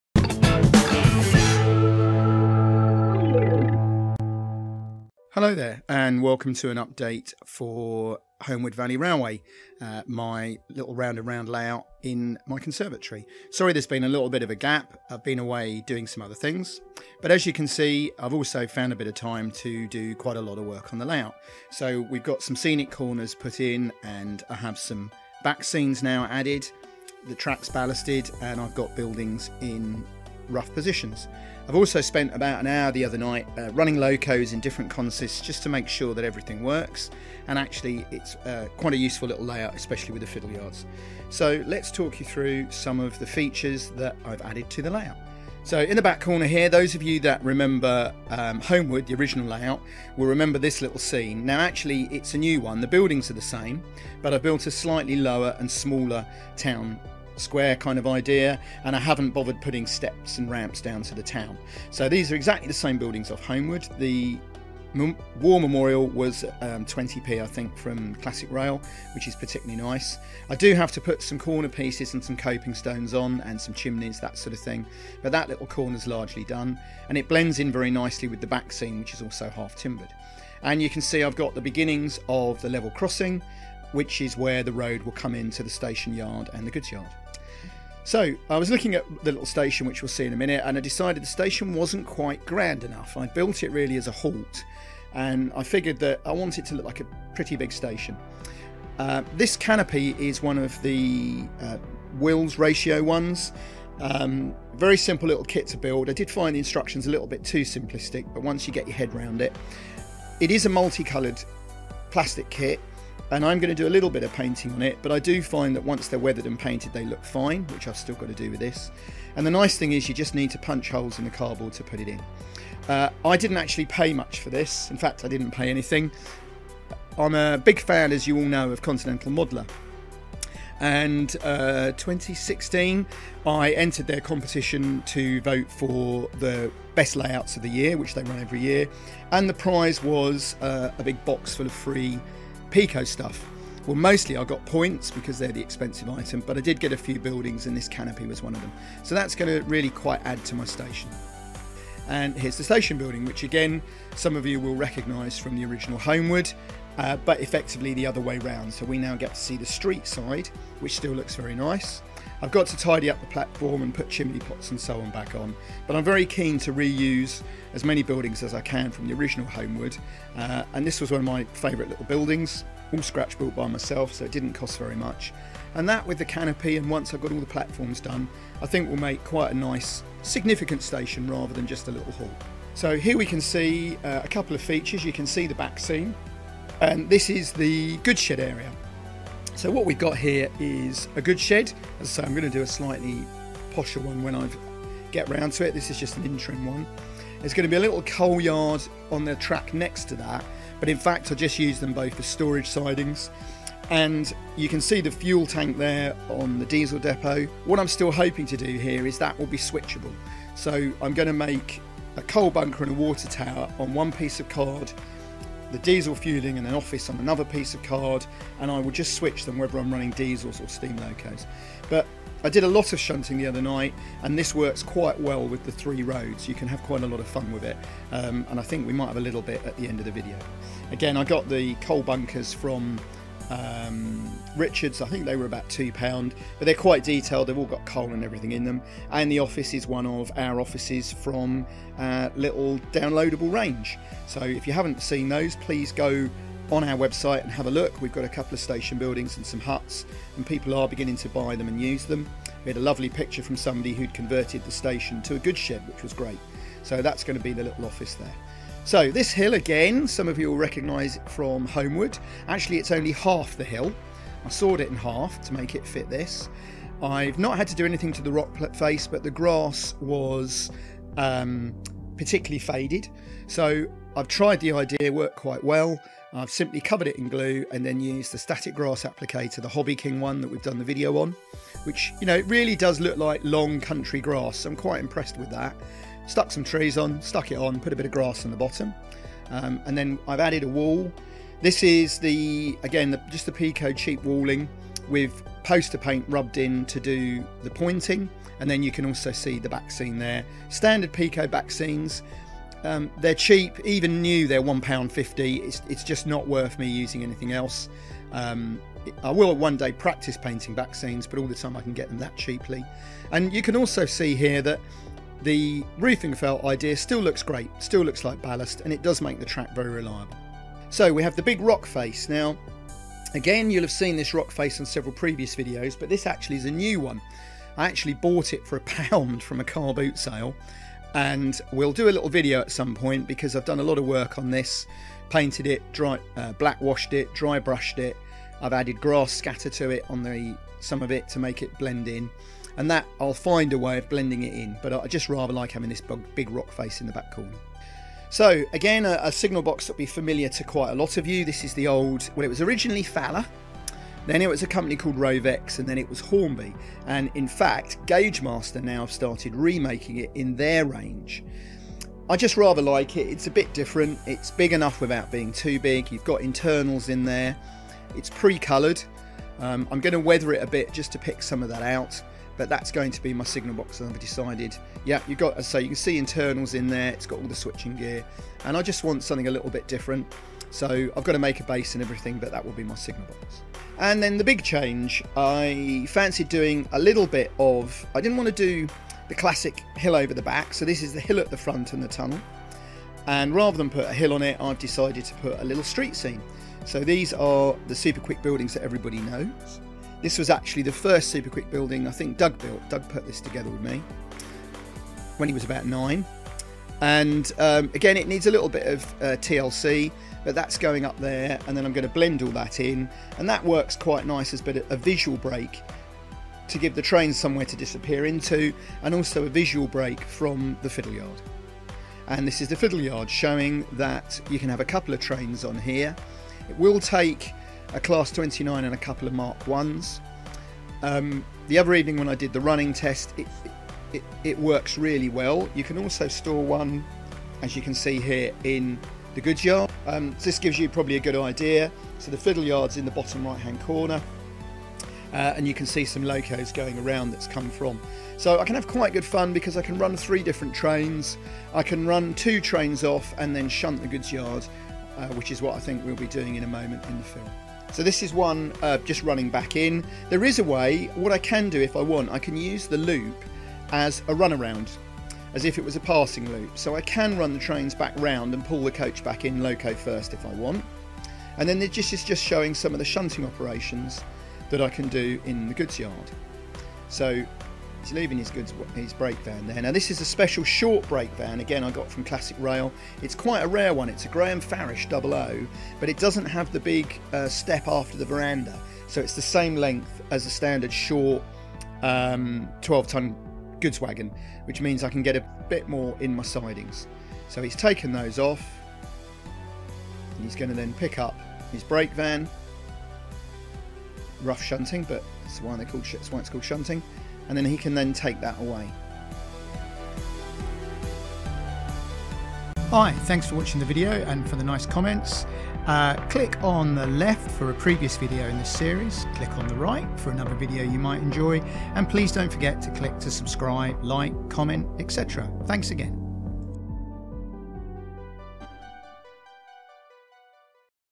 How To Build A Model Railway Layout - Goods Yard & More - Homewood Valley Railway 3
Kato chassis:
Hello And Welcome To Budget Model Railways, where you can learn how to make all aspects of a model railway cheaply!